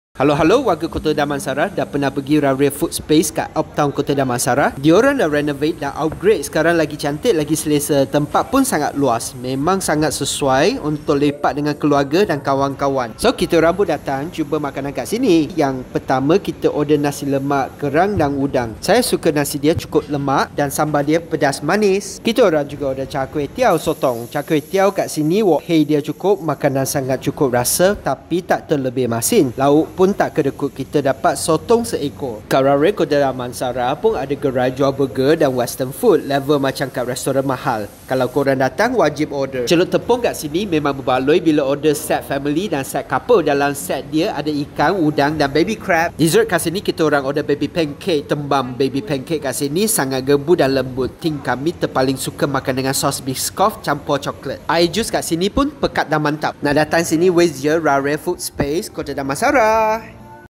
Food Court yang terpaling esthetic di Kota Damansara
Food court cantik macam kat taman denga pelbagai pilihan makanan Melayu, Western, Arab dan dessert. Minuman pun sedap.